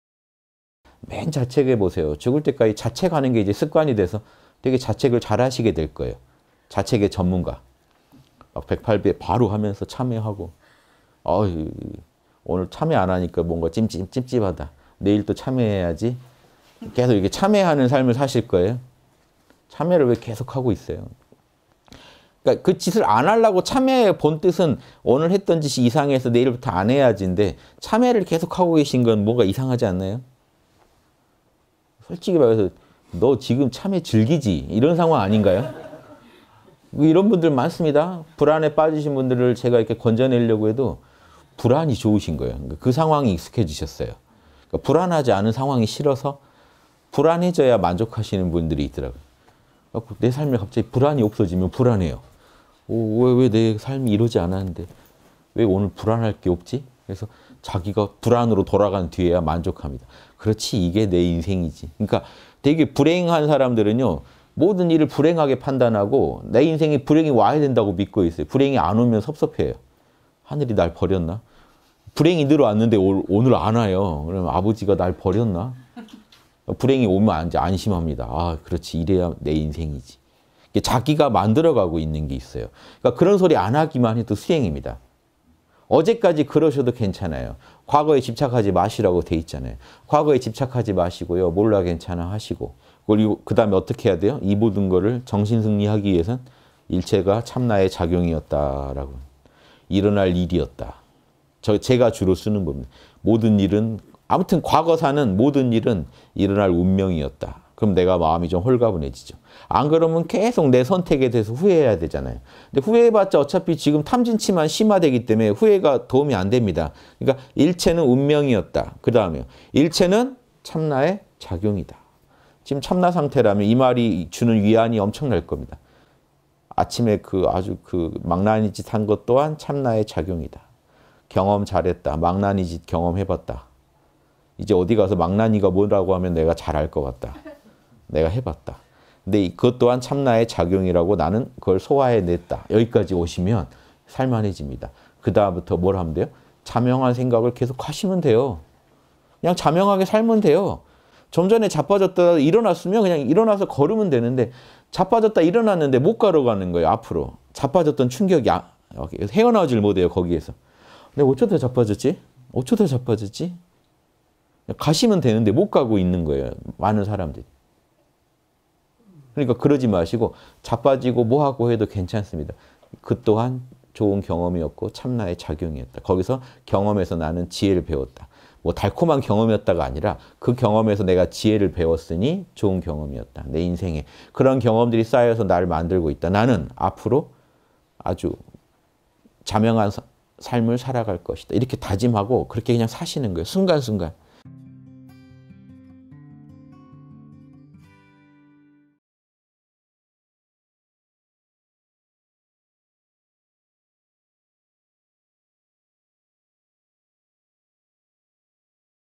[3분 양심] 나는 자책의 전문가 _홍익학당.윤홍식
(190306 수낮대담) 매일 자책 해보세요.
죽을 때까지 자책하는 게 이제 습관이 돼서
되게 자책을 잘하시게 될 거예요.
자책의 전문가.
108배 바로 하면서 참회하고.
“아이, 오늘 참회 안 하니까 뭔가 찜찜하다.”
“내일도 참회해야지.”
계속 이렇게 참회하는 삶을 사실 거예요?
참회를 왜 계속하고 있어요?
그러니까 그 짓을 안 하려고,
참회의 본뜻은
오늘 했던 짓이 이상해서
내일 부터 안 해야지 인데
참회를 계속하고 계신 건 뭔가 이상하지 않아요?
“솔직히 말해서, 너 지금 참회 즐기지?”
이런 상황 아닌가요?

뭐 이런 분들 많습니다.
불안에 빠지신 분들을 제가 건져내려고 해도
불안이 좋으신 거예요.
그 상황이 익숙해지셨어요.
불안하지 않은 상황이 싫어서
불안해져야 만족하신 분들이 있더라구요.
내 삶에 갑자기 불안이 없어지면 불안해요.
“어, 왜 내 삶이 이러지 않았는데
왜 오늘 불안할 게 없지?”
그래서 자기가 불안으로 돌아간 뒤에 만족합니다.
“그렇지 이게 내 인생이지.”
그러니까 되게 불행한 사람들은
모든 일을 불행하게 판단하고
내 인생에 불행이 와야 한다고 믿고 있어요.
불행이 안 오면 섭섭해요.

“하늘이 날 버렸나?”
불행이 늘 왔는데 오늘 안 와요.
그러면 “아버지가 날 버렸나?”
그래서 불행이 오면 안심합니다.
“아. 그렇지 이래야 내 인생이지.”
자기가 만들어 가고 있는 게 있어요.
그러니까 그런 소리 안 하기만 해도 수행입니다.

어제까지 그러셔도 괜찮아요.
과거에 집착하지 마시라고 되어 있잖아요.
과거에 집착하지 마시고,
몰라 괜찮아하시고
그리고 그다음에 어떻게 해야 돼요?
이 모든 것을 정신 승리하기 위해선
“일체가 참나의 작용이었다.” 라고.
“일어날 일이었다.”
제가 주로 쓰는 겁니다.
모든 일은, 아무튼 과거사는
모든 일은 일어날 운명이었다.

그러면 내가 마음이 좀 홀가분해지죠.
안 그러면 계속 내 선택에 대해서 후회해야 하잖아요.
그런데 후회해봤자 어차피 지금 탐진치(貪瞋癡)만
심화되기 때문에 후회가 도움이 안 됩니다.
그러니까 "일체는 운명이었다."
그 다음에요?
"일체는 참나의 작용이다."
지금 참나 상태라면 이 말이 주는 위안이 엄청날 겁니다.
“아침에 망나니짓 한 것 또한 참나의 작용이다.”
“경험 잘 했다.”
“망나니 짓 경험해 봤다.”
“이제 어디 가서 망나니가 뭐냐고 하면
내가 잘 알 것 같다.”“내가 해봤다.”
그런데 "그것 또한 참나의 작용이라고
나는 그걸 소화해 냈다." 이제 여기까지 오시면
살만해집니다. 그다음부터 뭘 하면 돼요?

자명한 생각을 계속 하시면 되요.
그냥 자명하게 살면 돼요.
좀 전에 자빠져다 일어났으면
그냥 일어나서 걸으면 되는데
자빠졌다 일어났는데 못 걸어가는 거예요. 앞으로.
자빠졌던 충격에서 헤어나지 못해요, 거기에서.
"왜 자빠졌지?" "어쩌다 자빠졌지?"
그냥 가시면 되는데 못 가고 있는 거예요,
많은 사람들이.

그러니까 그러지 마시고
자빠지고 뭐하고 해도 괜찮습니다.
그 또한 좋은 경험이었고,
참나의 작용이었다. 거기서
경험에서 나는 지혜를 배웠다.
뭐 달콤한 경험이었다가 아니라
그 경험에서 내가 지혜를 배웠으니
좋은 경험이었다, 내 인생에서.
그런 경험들이 쌓여서 날 만들고 있다.
나는 앞으로
아주 자명한 삶을 살아갈 것이다.
이렇게 다짐하고 그냥 그렇게 사시는 거예요. 순간순간.